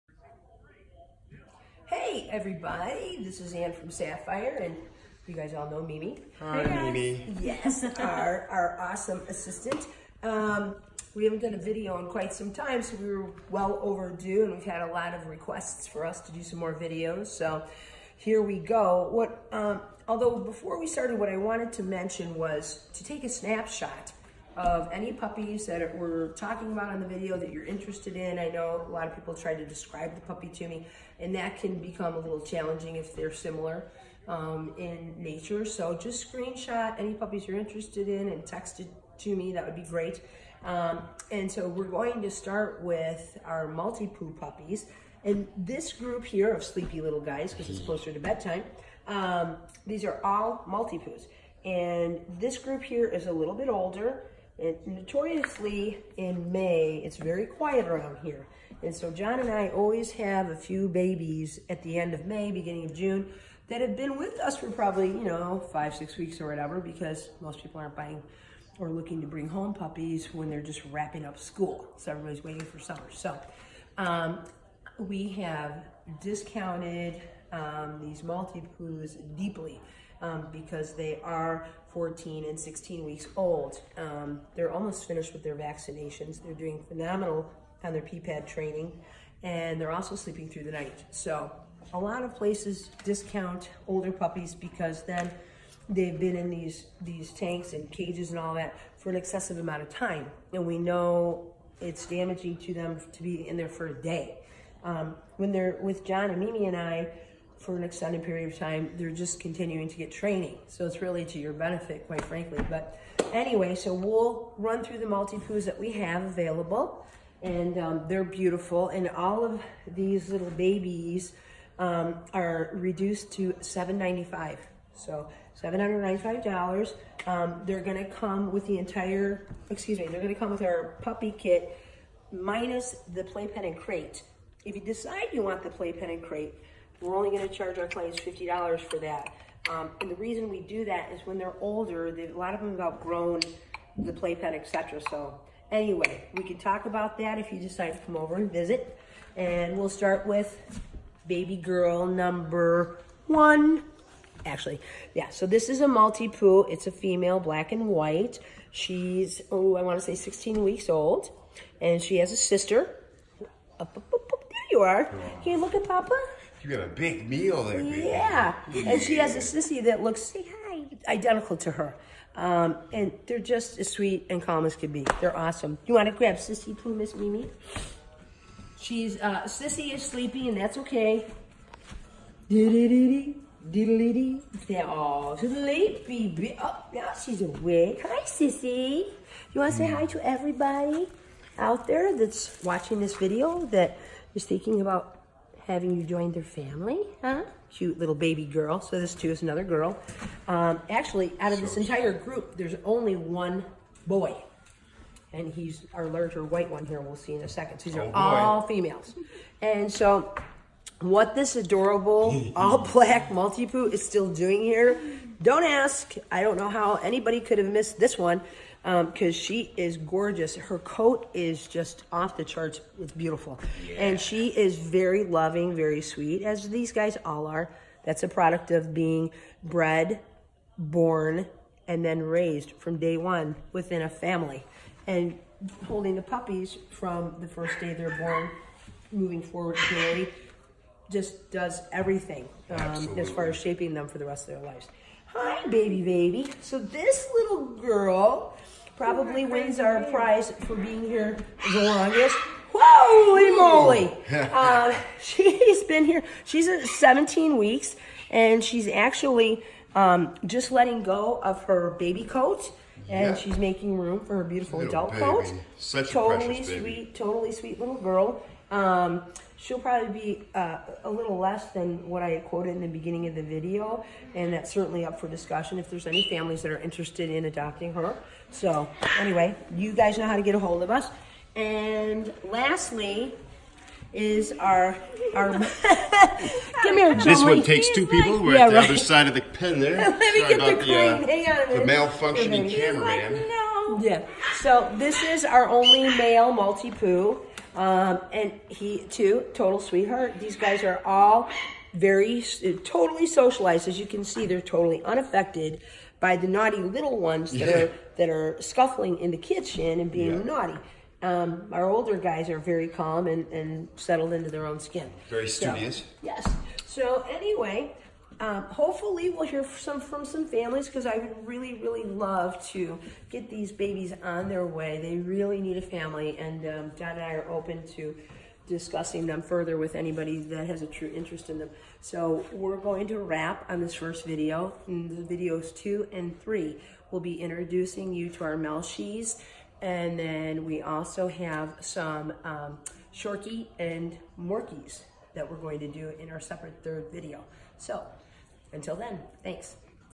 MALTIPOO’S AVAILABLE A/O 5/29/19
Currently, we have four girls and one
boy!!

2 girls black & white
1 girl all white
1 boy that's also all white

YES We are still including the puppy kit!😃